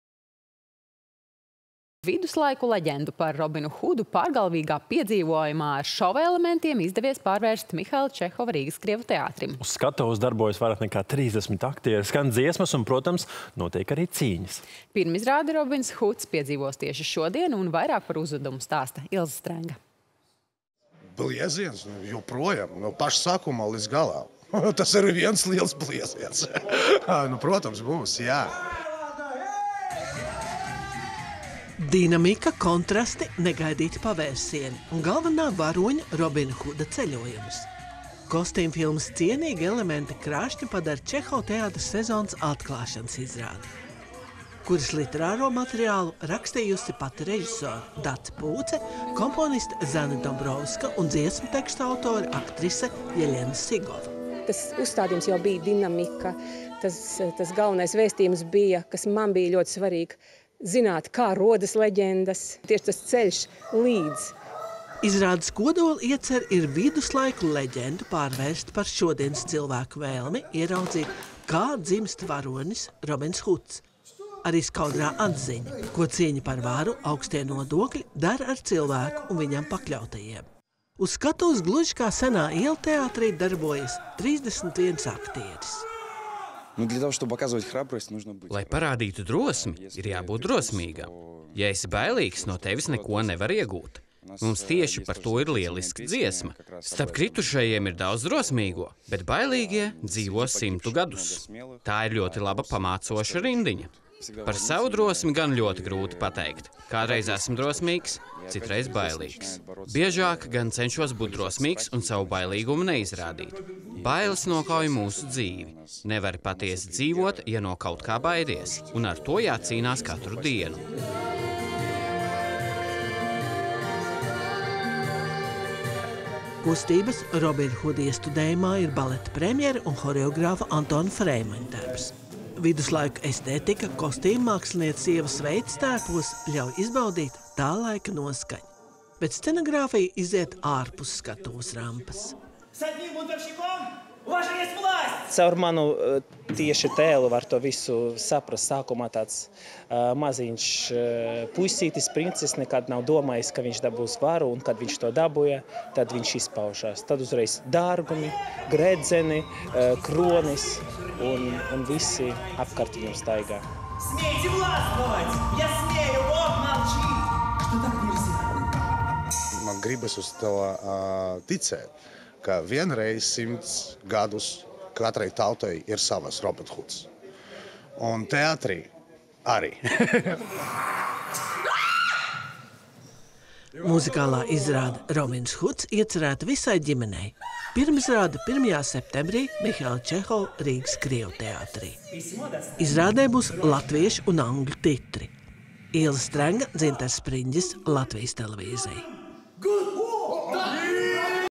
Viduslaiku leģenda “Robins Huds” pārgalvīgs piedzīvojums
Viduslaiku leģendu  par Robinu Hudu  pārgalvīgā piedzīvojumā izdevies īsteno Mihaila Čehova Rīgas Krievu teātrim.
     Izrādes literāro materiālu Dace Pūce, Zane Dombrovska un Jeļena Sigova jau sākot darbu rakstīt, apzinājās, ka tas būs muzikāls iestudējums ar daudziem šova elementiem.
  Uz teātra skatuves darbojas vairāk nekā trīsdesmit aktieri, skan dziesmas, notiek cīņas un krāšņajā scenogrāfes Ievas Kauliņas, vidomākslinieka Viktora Keino un gaismu mākslinieka Mārtiņa Feldmaņa  uzburtajā pasaulē uznāk jaunais varonis – Robins Huds.
   Muzikālās izrāde “Robins Huds” pirmizrāde  1.septembrī.